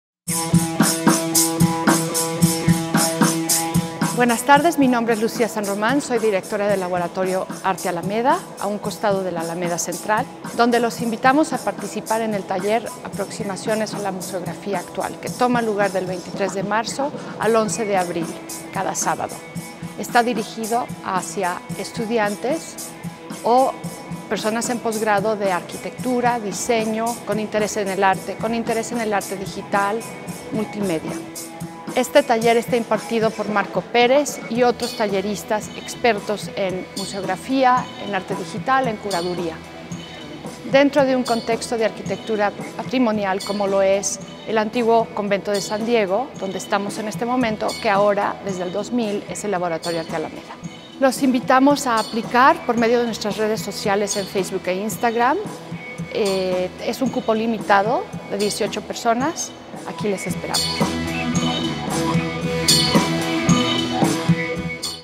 Segundo Taller "Arte y Tecnología 2019" en el Laboratorio Arte Alameda
Del 23 de marzo al 22 de junio de 2019
Sábados de 10 a 14h

Proyecto realizado por WIKIA en colaboración con el Laboratorio Arte Alameda